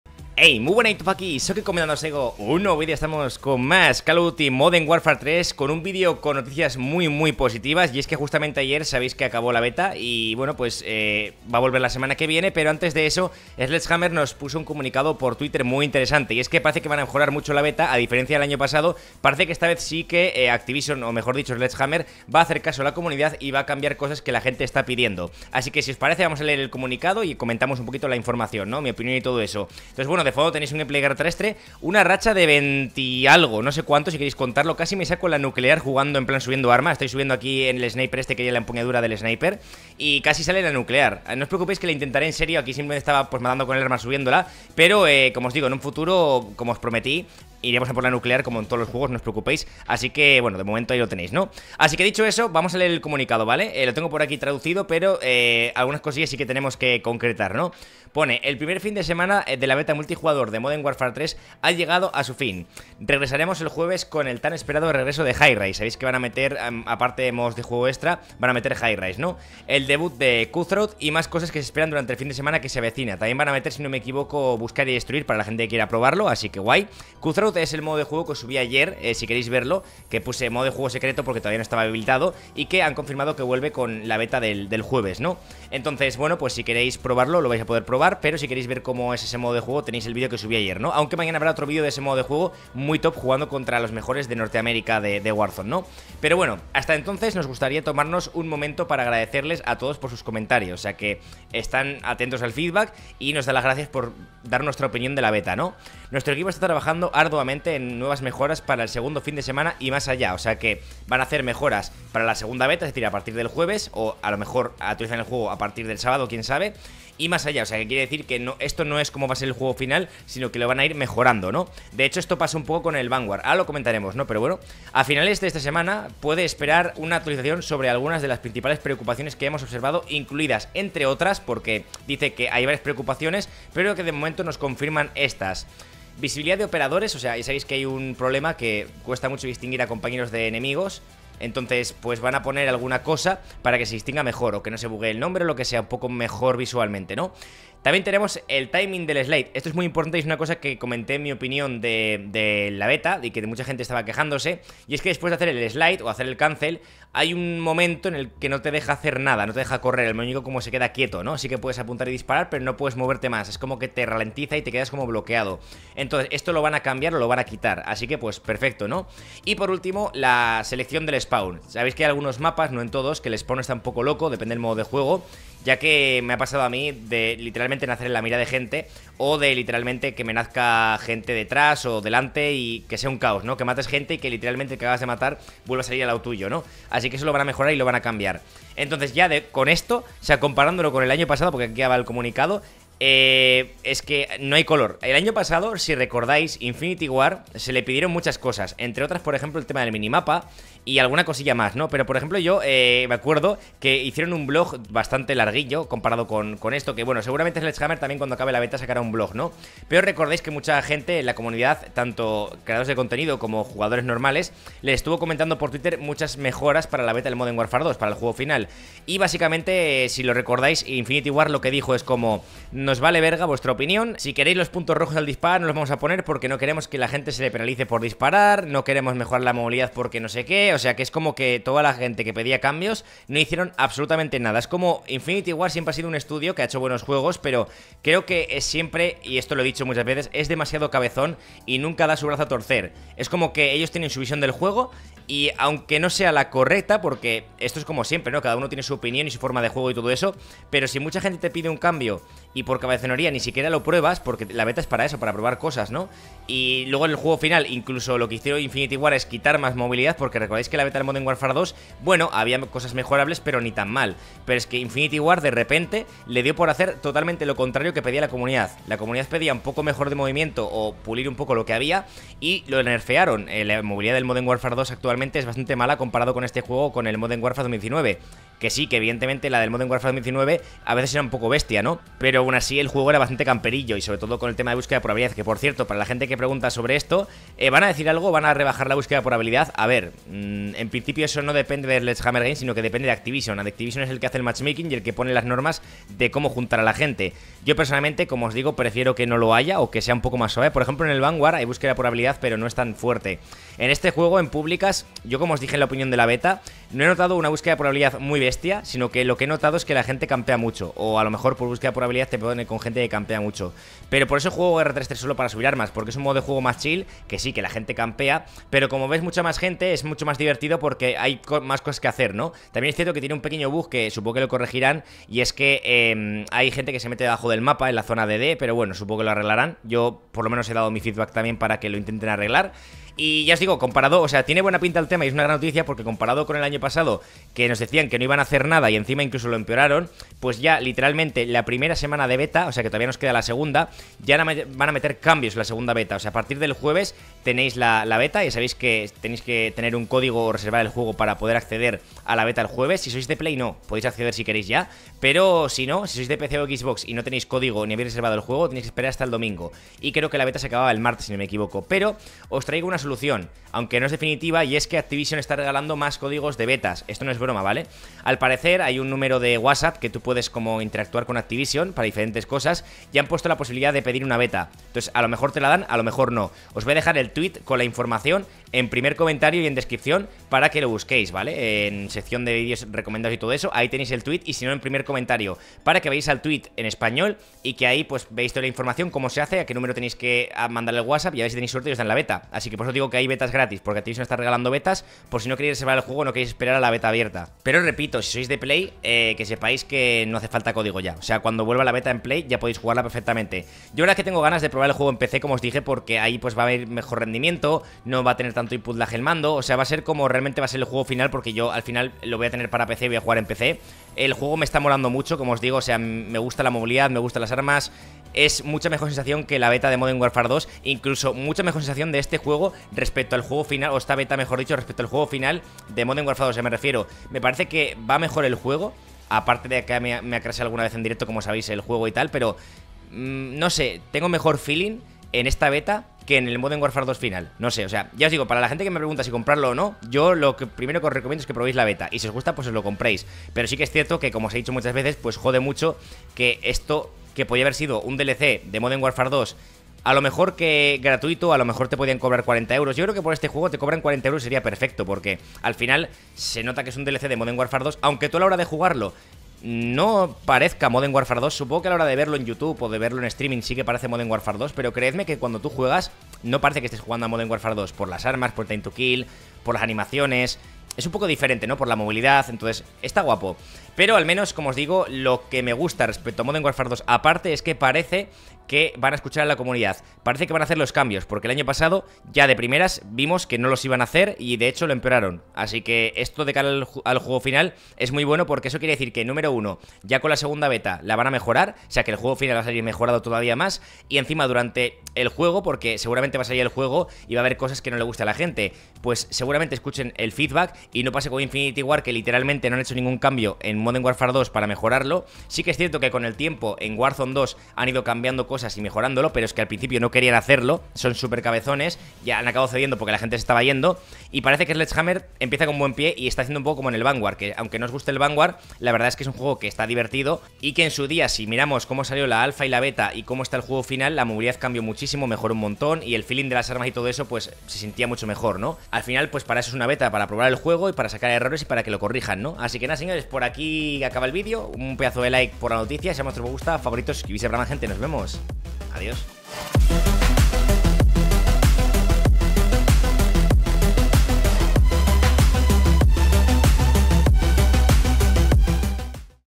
La MEJOR NOTICIA para COD MODERN WARFARE 3 + BETA GRATIS
Buenas gente! EN ESTE VIDEO OS TRAIGO LA MEJOR NOTICIA POSIBLE PARA CALL OF DUTY MODERN WARFARE 3 BETA! NOS ESCUCHAN! Y YA VAN A CAMBIAR LO QUE PEDIMOS Y A MEJORAR EL JUEGO!
Espero que os guste! Like y sub para mas! :D

🎧 Utilizo unos auriculares JBL Quantum One! Aqui teneis todos los modelos!